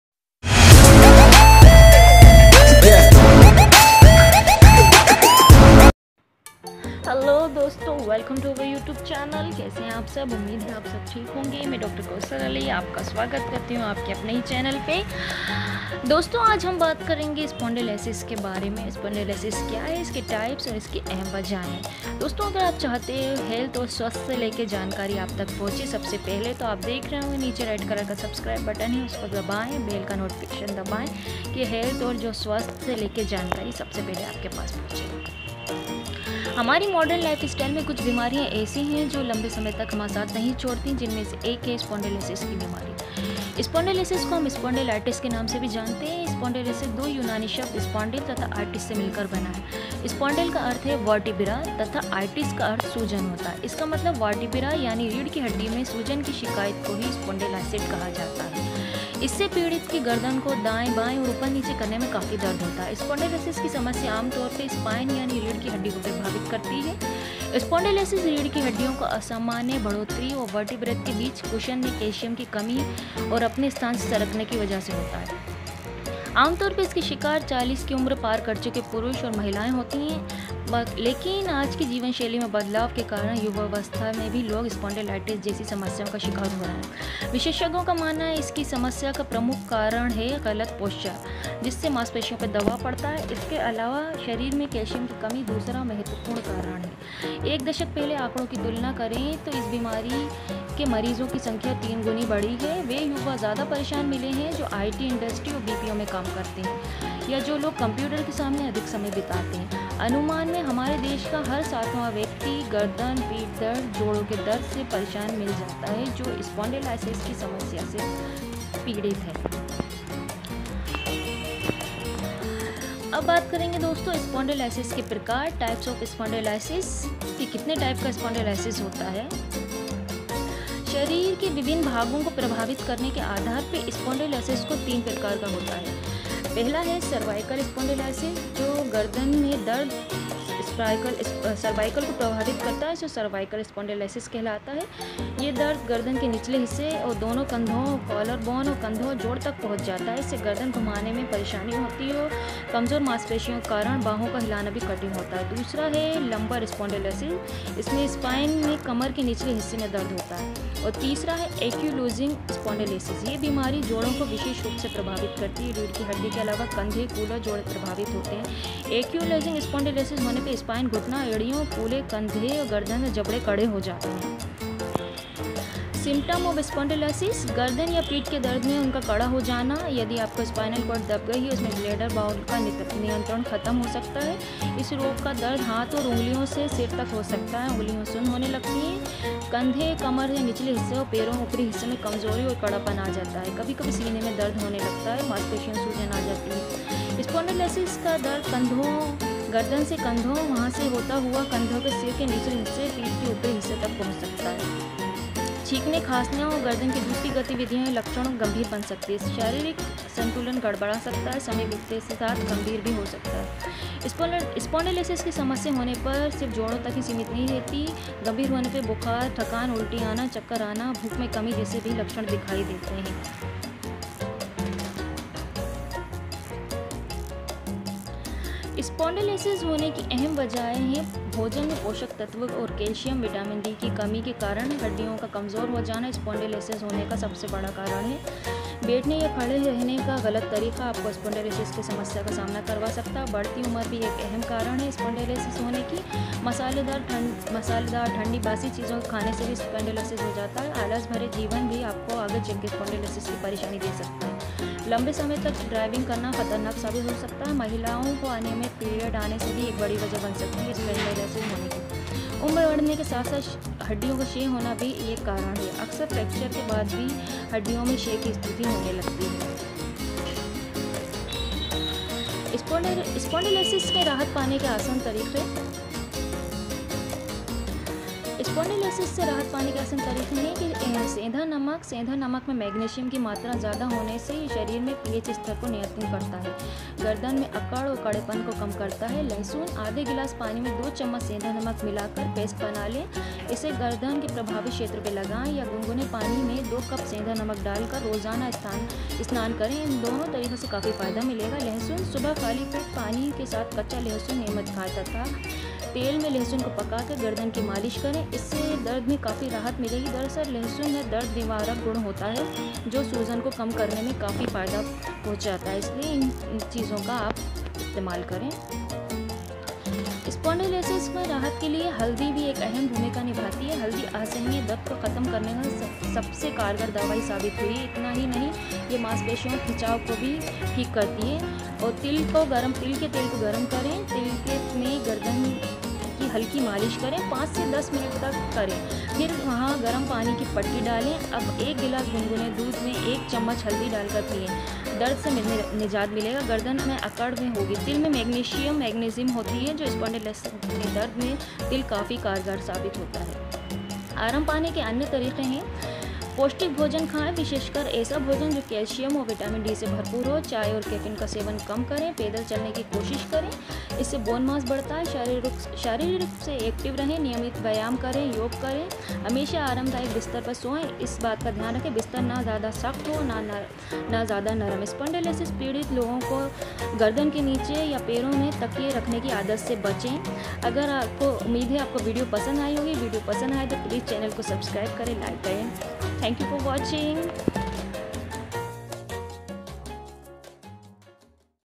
👉Spondylitis In Hindi - जाने क्या है स्पोंडिलोसिस की बीमारी Home remedies..
हमारी मॉडर्न लाइफस्टाइल में कुछ बीमारियां ऐसी हैं जो लंबे समय तक हमारा साथ नही छोड़ती। जिनमें से एक है स्पोंडिलोसिस की बीमारी। स्पोंडिलोसिस को हम स्पॉन्डिलाइटिस के नाम से भी जानते है।स्पोंडिलोसिस दो यूनानी शब्द ‘स्पॉन्डिल’ तथा ‘आइटिस’ से मिलकर बना है। स्पॉन्डिल का अर्थ है वर्टिब्रा तथा ‘आइटिस’ का अर्थ सूजन होता है इसका मतलब वर्टिब्रा यानी रीढ़ की हड्डी में सूजन की शिकायत को ही स्पॉन्डिलाइटिस कहा जाता है। इसमें पीड़ित को गर्दन को दाएं- बाएं और ऊपर-नीचे करने में काफी दर्द होता है। स्पोंडिलोसिस की समस्या आम तौर पे स्पाइन यानी रीढ़ की हड्डी को प्रभावित करती है। स्पोंडिलोसिस रीढ़ की हड्डियों की असामान्य बढ़ोत्तरी और वर्टेबट के बीच के कुशन में कैल्शियम की कमी और अपने स्थान से सरकने की वजह से होता है। dr education
आमतौर पर इसके शिकार 40 की उम्र पार कर चुके पुरुष और महिलाएं होती हैं। आज की जीवनशैली में बदलाव के कारण युवावस्था में ही लोग स्पॉन्डिलाइटिस जैसी समस्याओं के शिकार हो रहे हैं। विशेषज्ञों का मानना है कि इस समस्या का सबसे प्रमुख कारण गलत पॉश्चर है, जिससे मांसपेशियों पर दबाव पड़ता है। इसके अलावा शरीर में कैल्शियम की कमी दूसरा महत्वपूर्ण कारण है। एक दशक पहले के आंकड़ों से तुलना करें तो इस बीमारी के मरीजों की संख्या तीन गुनी बढ़ी है। वे युवा ज्यादा परेशान मिलते हैं, जो आईटी इंडस्ट्री या बीपीओ में काम करते हैं या जो लोग कम्प्यूटर के सामने अधिक समय बिताते हैं। अनुमानतः हमारे देश का हर सातवाँ व्यक्ति गर्दन और पीठ दर्द या जोड़ों के दर्द से परेशान लोग मिल जाते हैं।